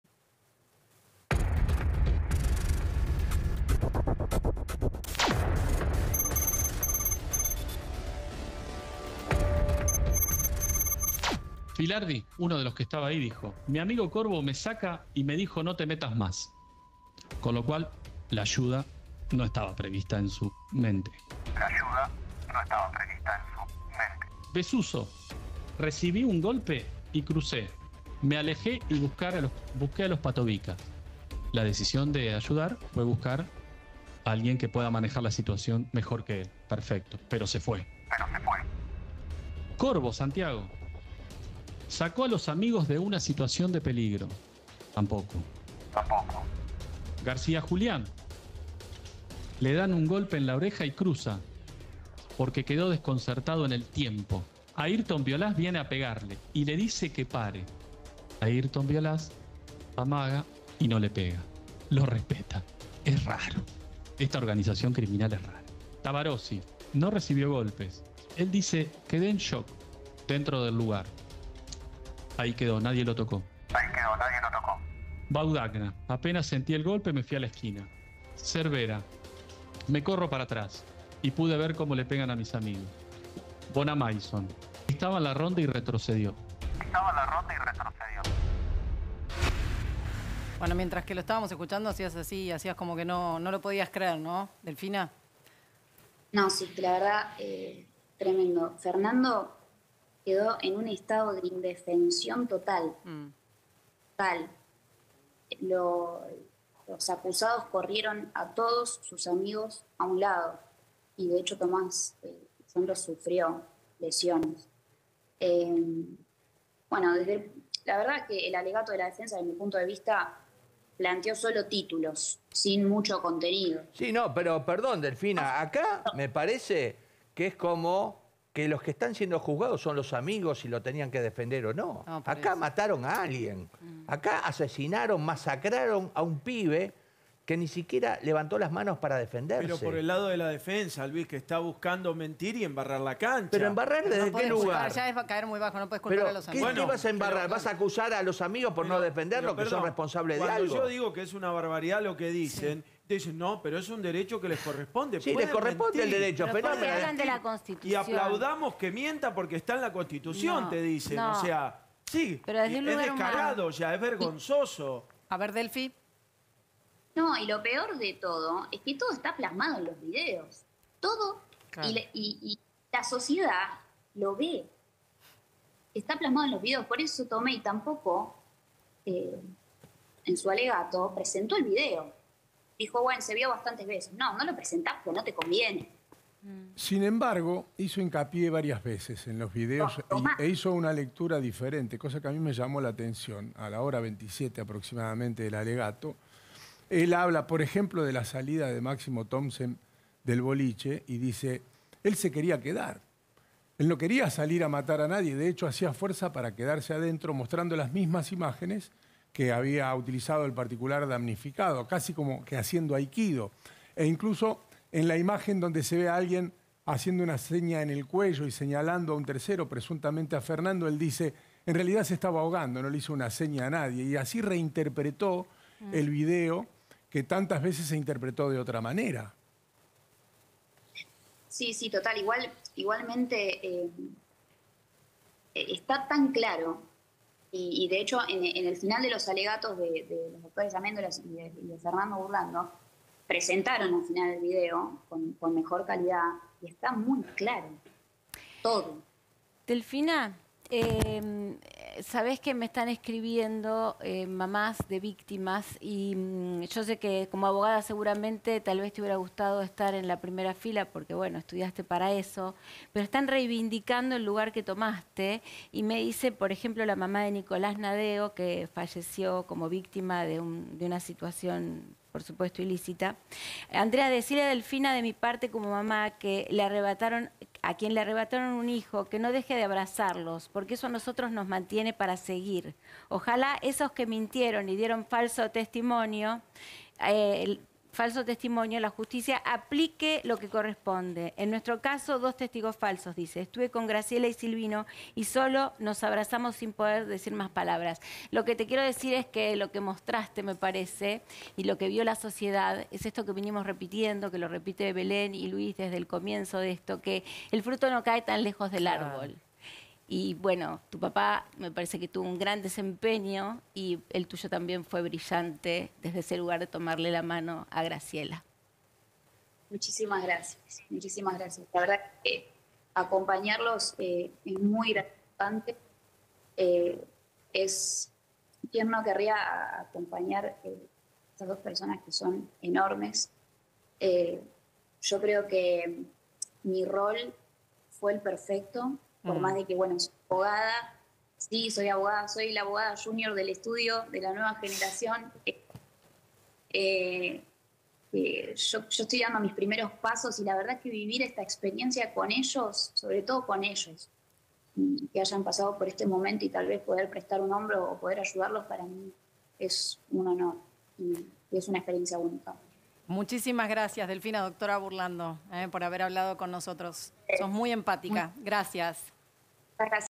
Delfina Burlando habló sobre los alegatos de la defensa de los rugbiers
Polémica por la defensa de los acusados por el crimen de Fernando Báez Sosa.